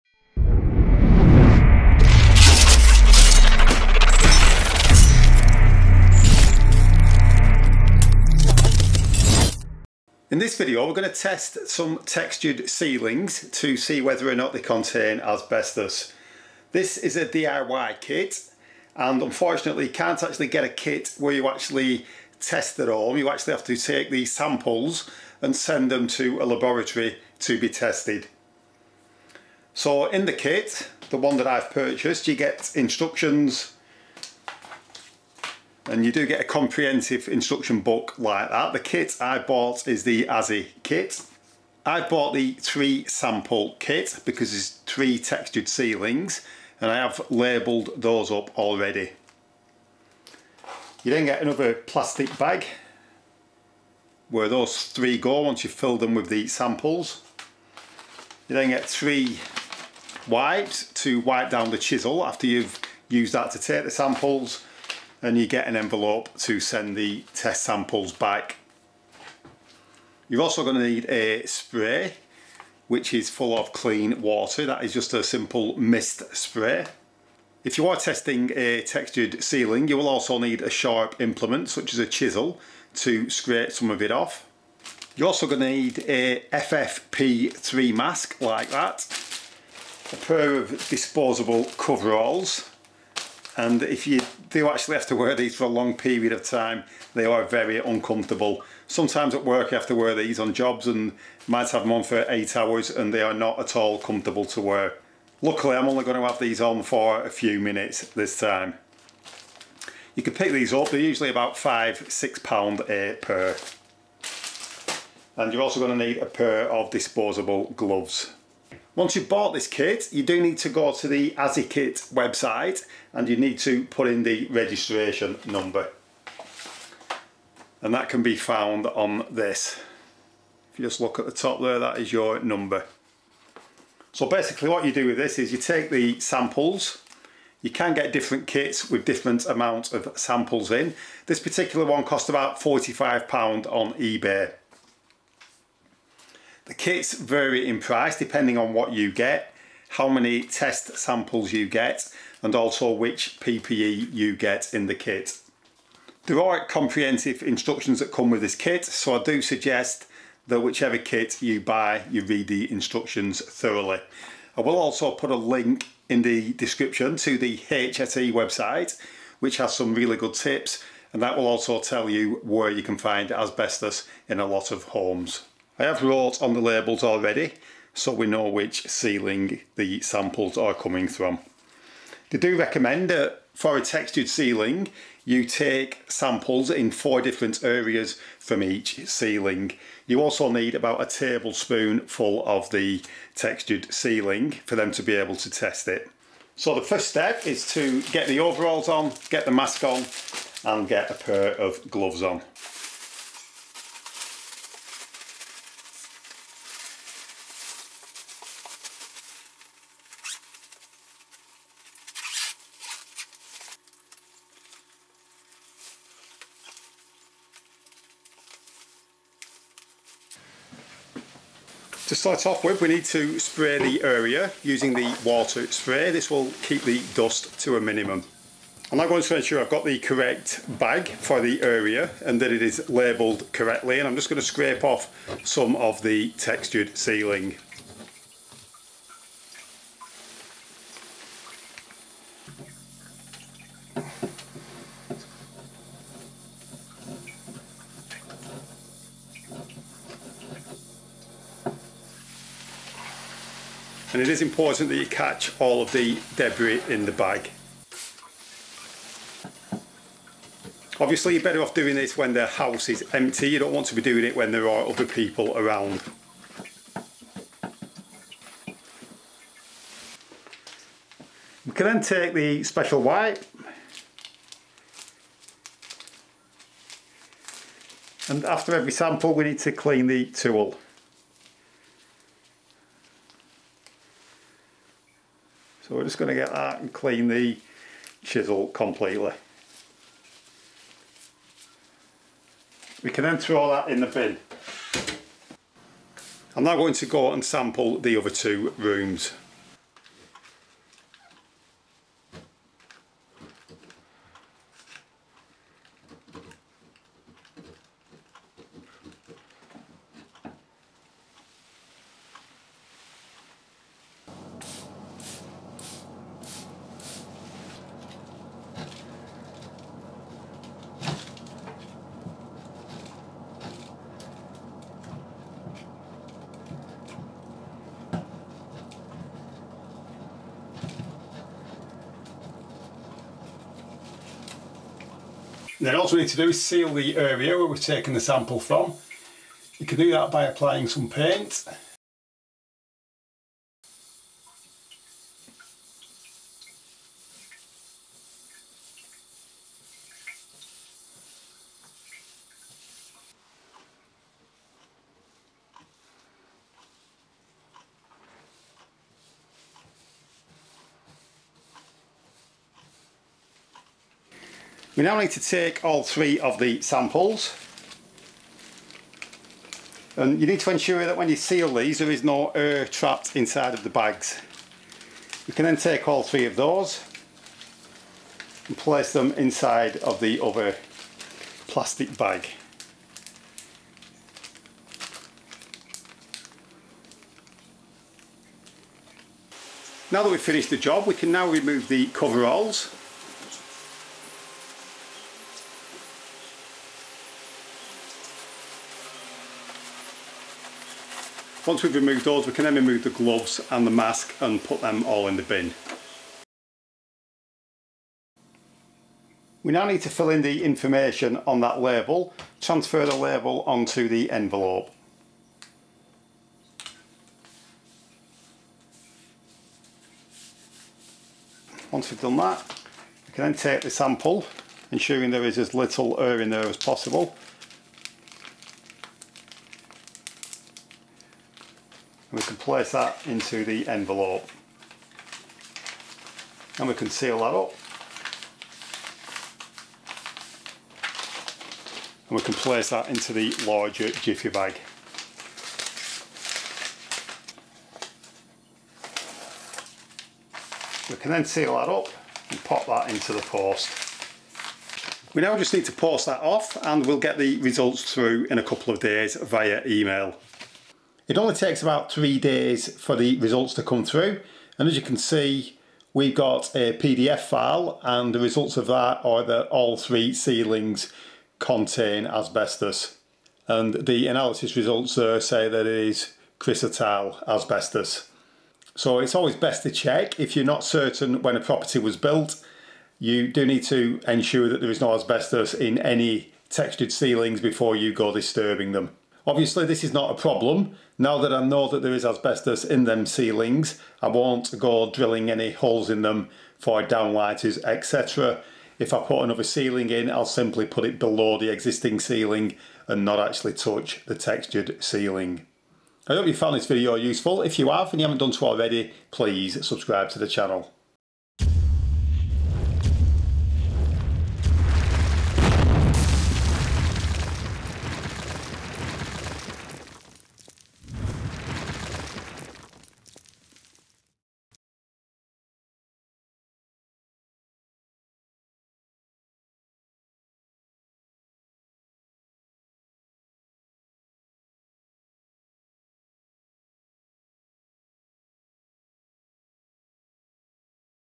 DIY asbestos testing
Asbestos can be found in many homes and surprisingly it was still being used in building products up until about the year 2000, even though it is common knowledge that asbestos causes lung disease including mesothelioma for decades. Asbestos can be found in lots of items around the home including floor tiles, asbestos soffits and textured ceilings. In this video I test three textured ceilings for asbestos using a test kit from azzykit. Textured ceilings containing asbestos are not a problem, as long as you leave them alone and do not go cutting into them or drilling them.

The HSE has some good information which you might find useful